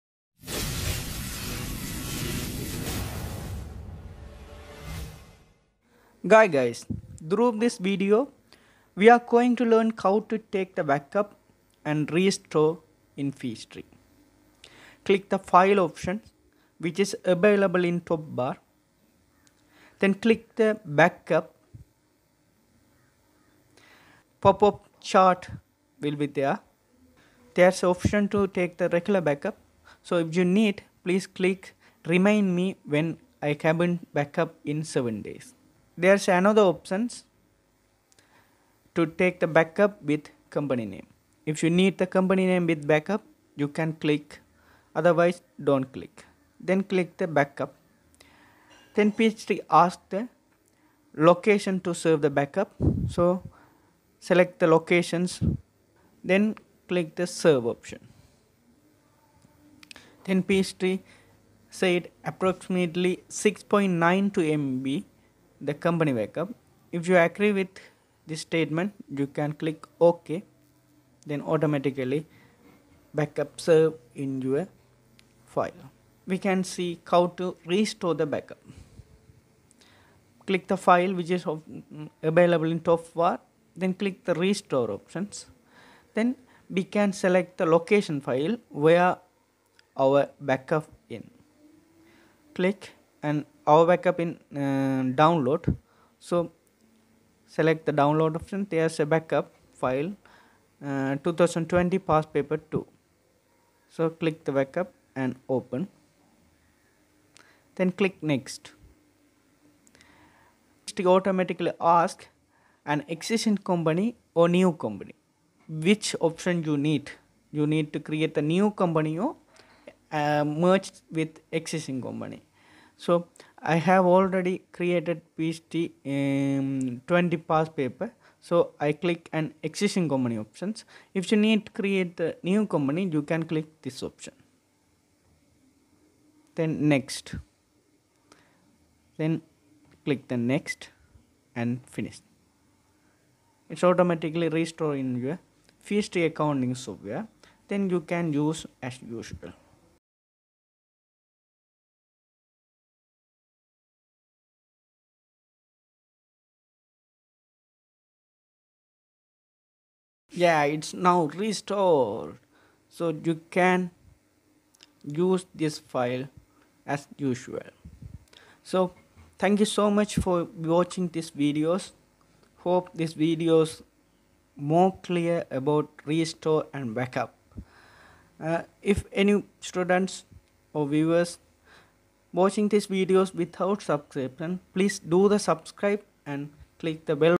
How to take | BACK UP & RESTORE | in Peachtree Accounting software
This video will describe that how to take the back up and restore the back up in Peachtree Accounting software.